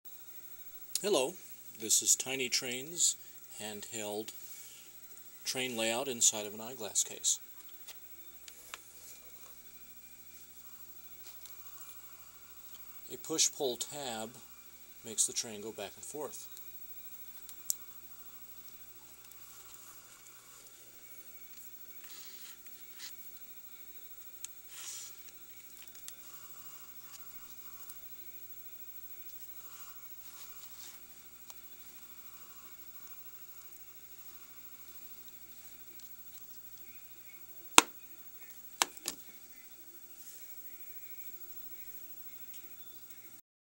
Tiny Trains Model Railroad in a Glasses Case
Tiny Trains Glasses Case Layout in 1:900 TY scale.
A miniature model railroad inside a glasses case!
A push-pull tab makes the train go back and forth.